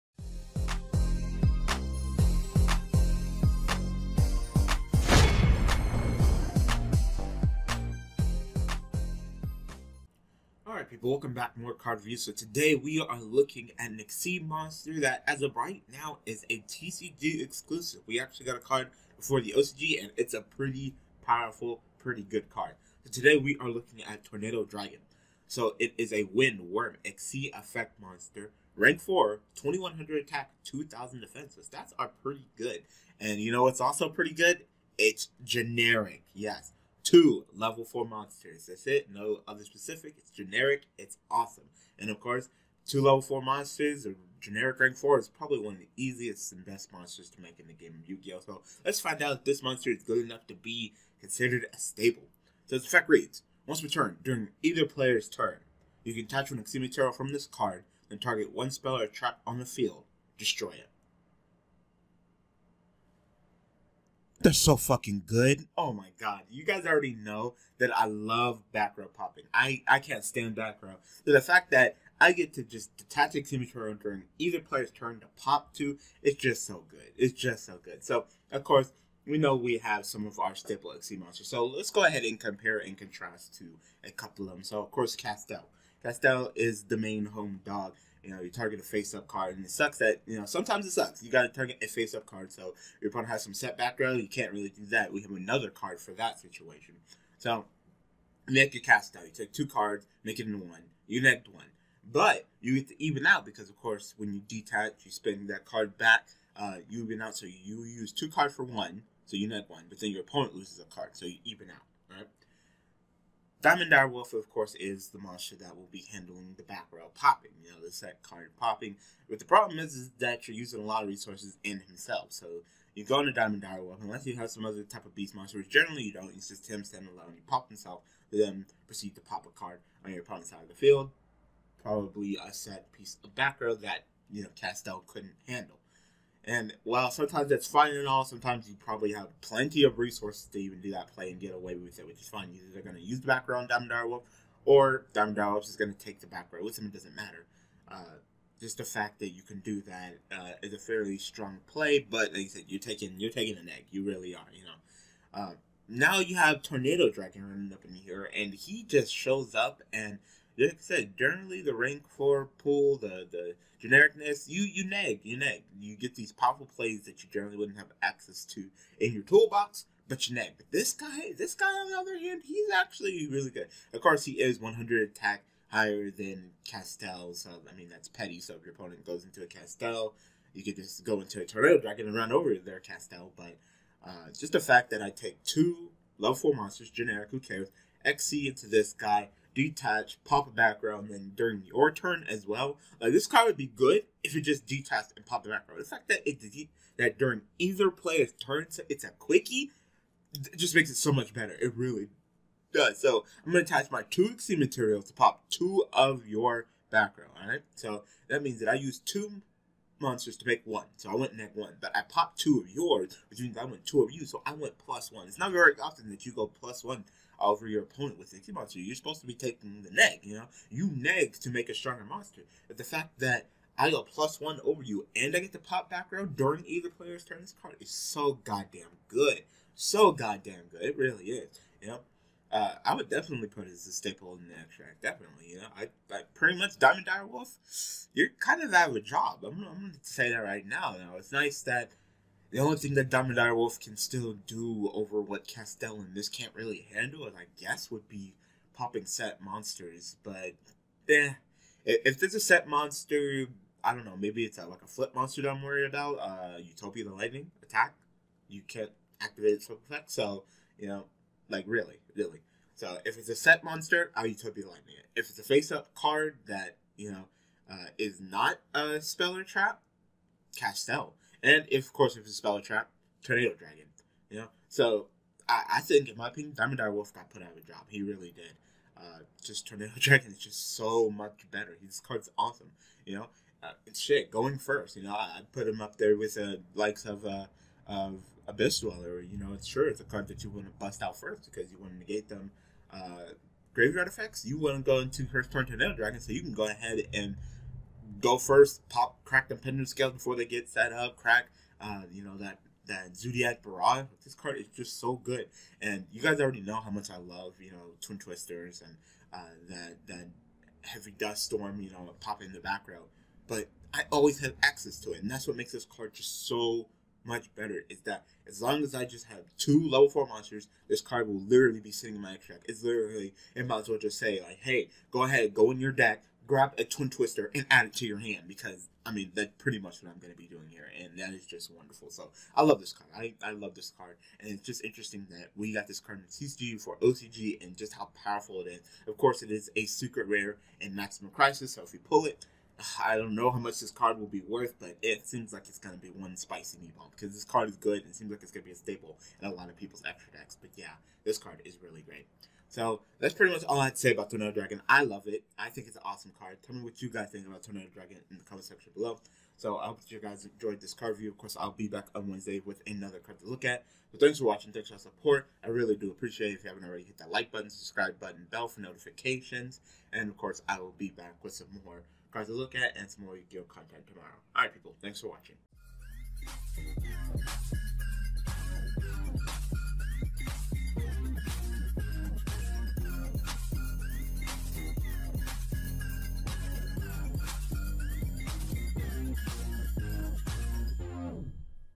Yubel Mastery Reviews: Tornado Dragon [Card Reviews]
Tornado Dragon
Wind / Rank 4 / Wyrm / XYZ / Effect / 2100 ATK 2000 DEF
2 Level 4 monsters
Once per turn, during either player’s turn: You can detach 1 Xyz Material from this card, then target 1 Spell/Trap Card on the field; destroy it.